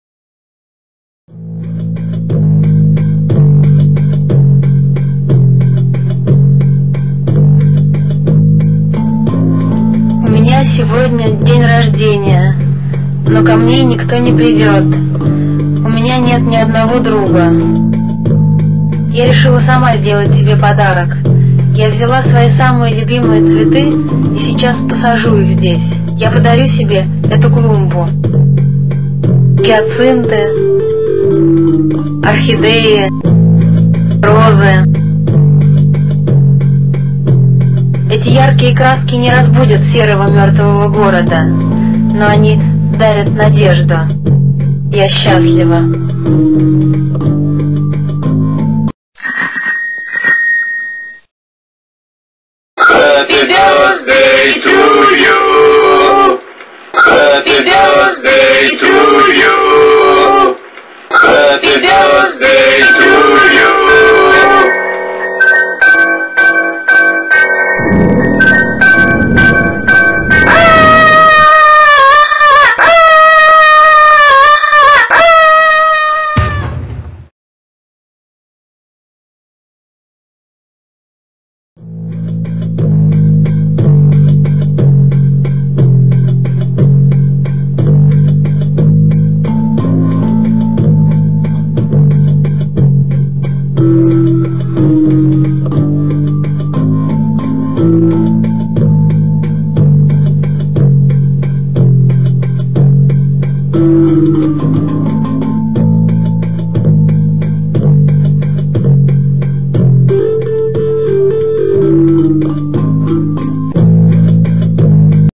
[HD] KOMS | Scary dolls | Куклы-уроды - С днем рождения 2002
It's my very first animated film. I just couldn't stop after that.
Мой самый первый мультик. Почему-то оказалось, что его здесь до сих пор нет...

Amotillado Андрей Бахурин